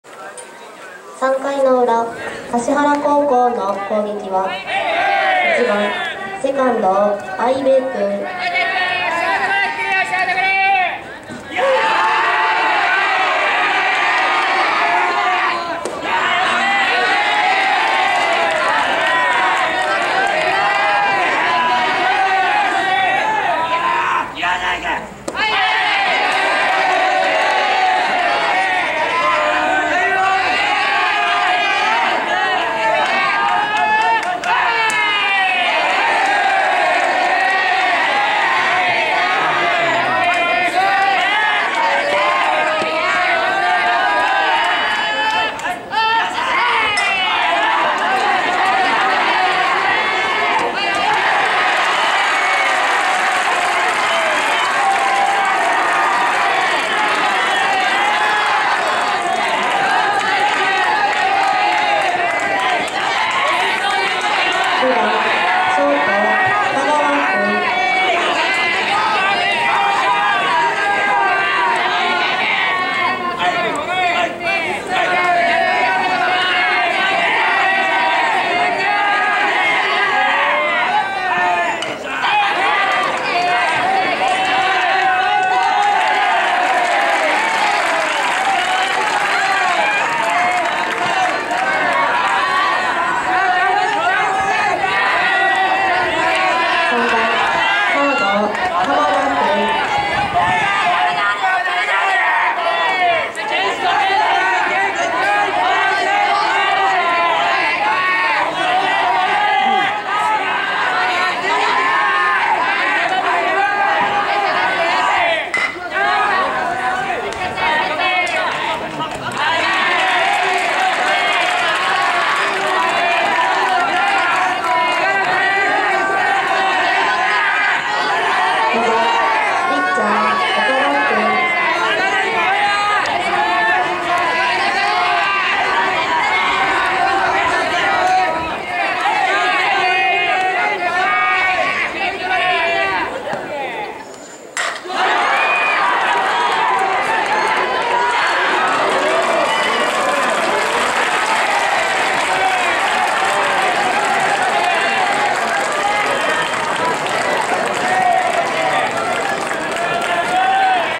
2015/9/13 関西中央vs橿原 ３回裏 橿原
奈良県高校野球秋季大会２回戦＠佐藤薬品スタジアム
関西中央vs橿原
３回裏　橿原
関西中央０－０橿原